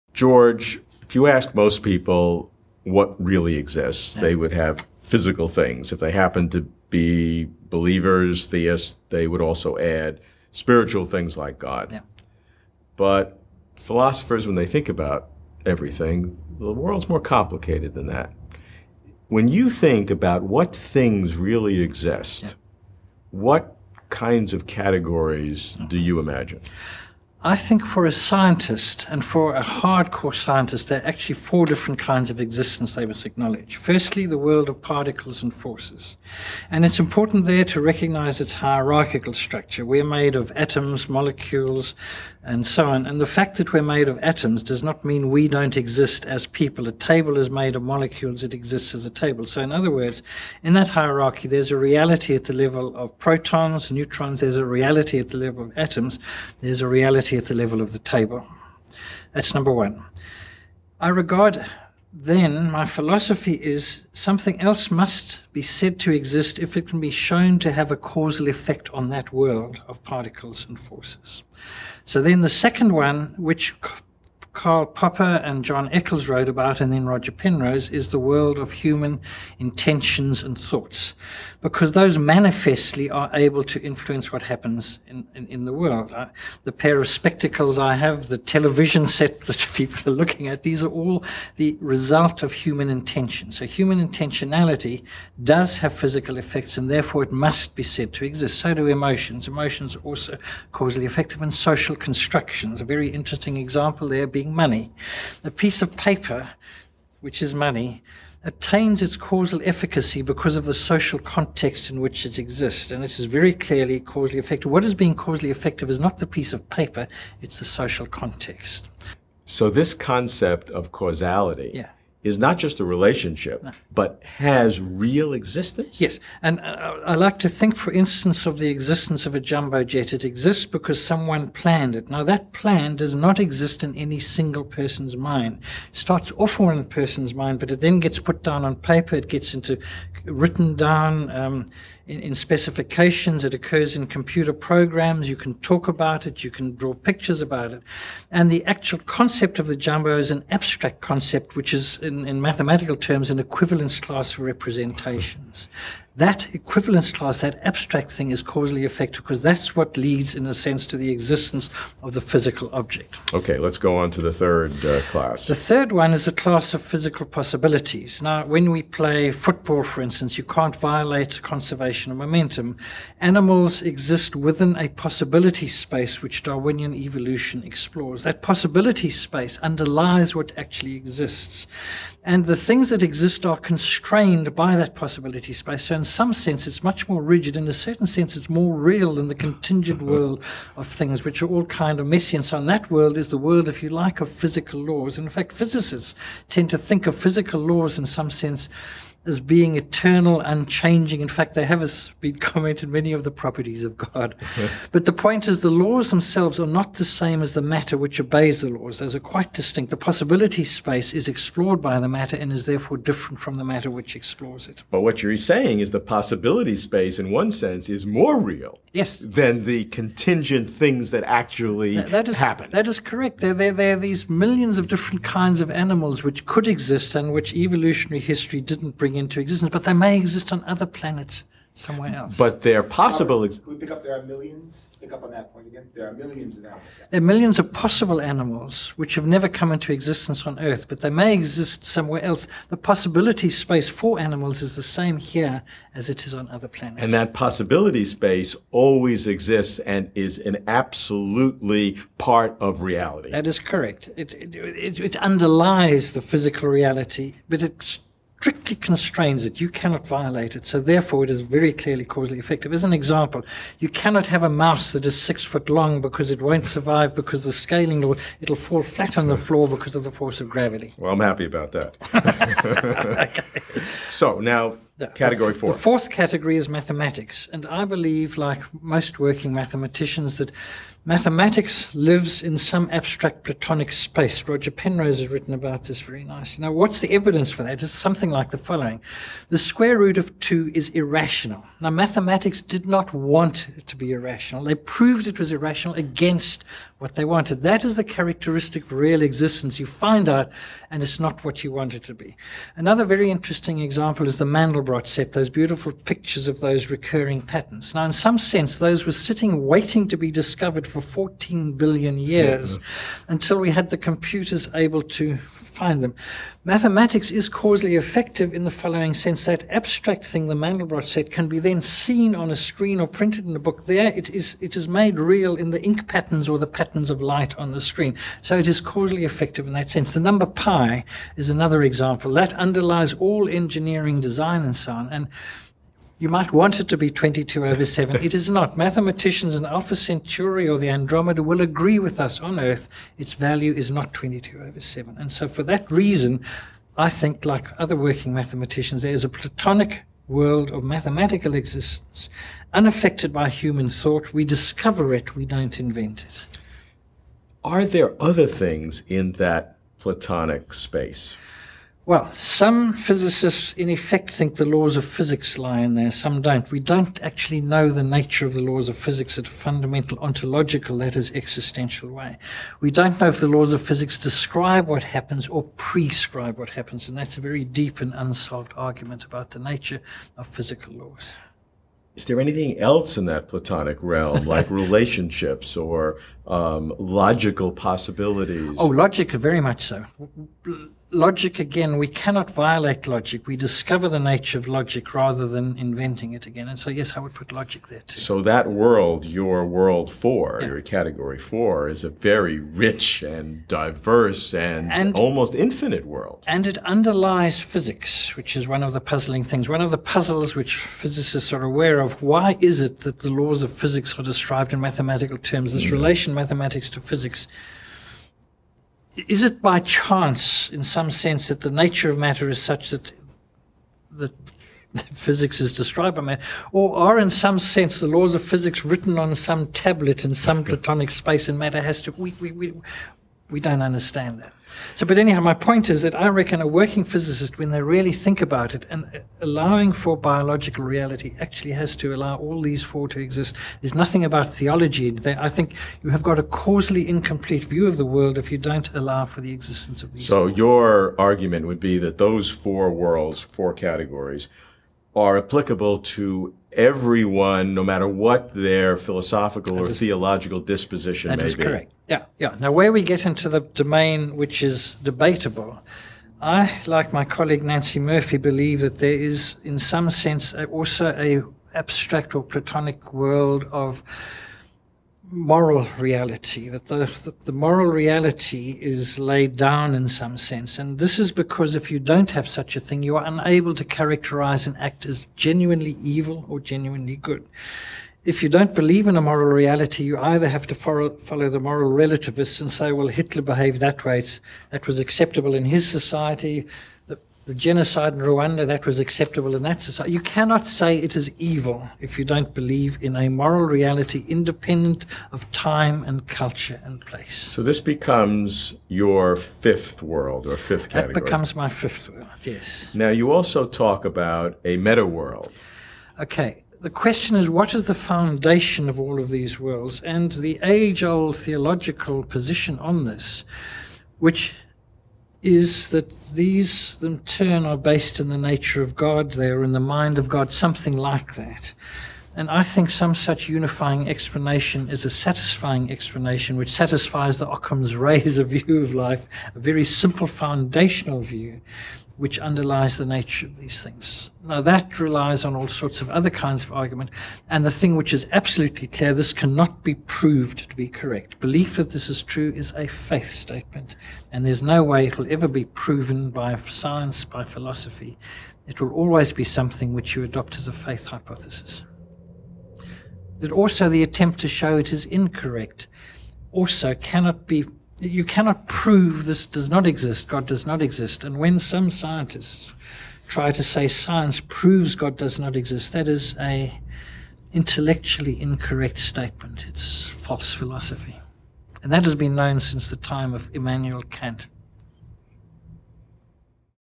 George Ellis - What Things Really Exist?
When you ask what things really exist, and you think deeply about this probe to apprehend what is out there, you see the whole world anew. What are the most general categories to understand the world? It’s such a simple question; how can it inspire such profound insights?

George F. R. Ellis is the Emeritus Distinguished Professor of Complex Systems in the Department of Mathematics and Applied Mathematics at the University of Cape Town in South Africa.